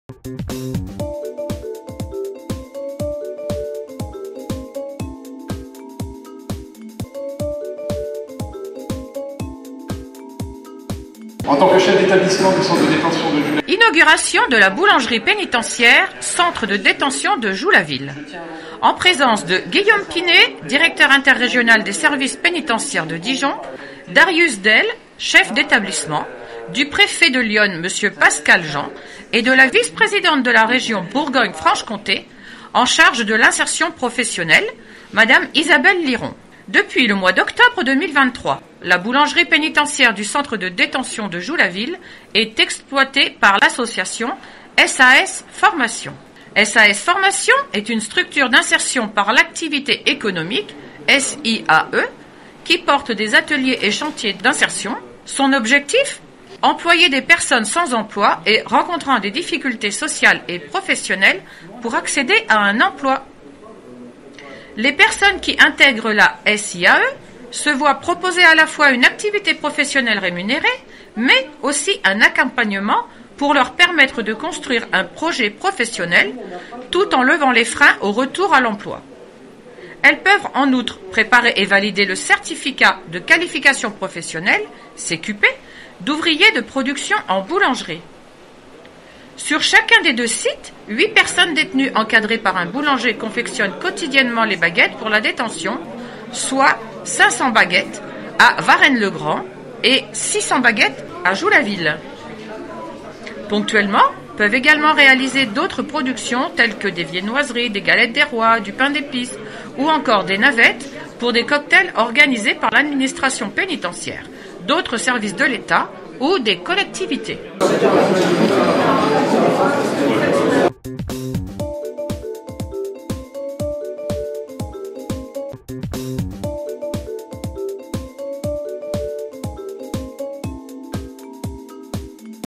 Inauguration du chantier d'insertion boulangerie du centre de détention de Joux-la-Ville.
Un projet qui s’inscrit pleinement dans l’objectif de dynamisation et de diversification du travail en milieu pénitentiaire pour lutter contre la récidive.